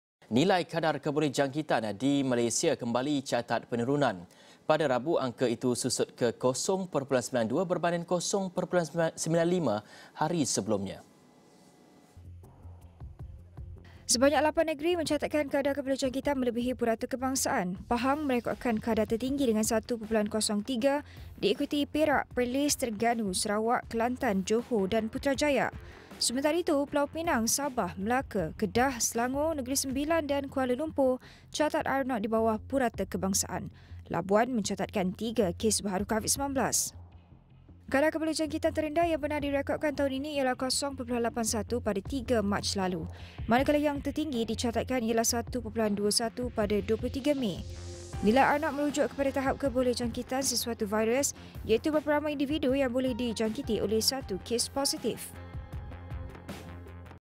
COVID-19 | Nilai RT/RO: Turun ke 0.92, Pahang tertinggi
Nilai kadar kebolehjangkitan COVID-19 di Malaysia kembali catat penurunan ke 0.92.